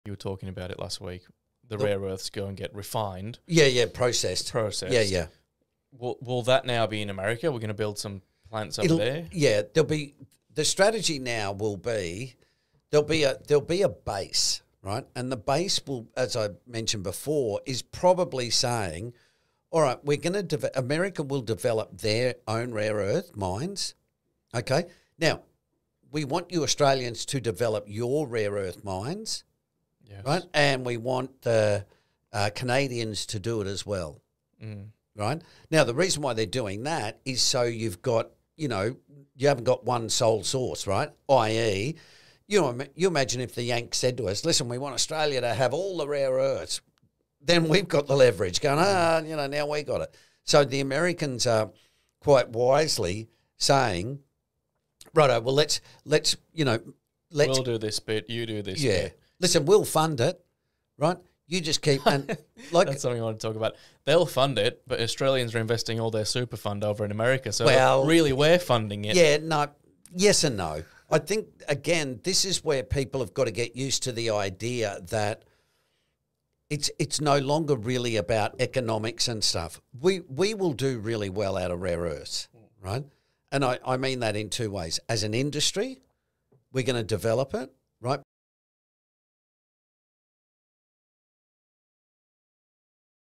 Rare Earths: America's Plan and Australia's Role
Why is America so interested in rare earth mining? Diversifying sources avoids reliance on a single country, giving no one nation too much leverage in this critical sector.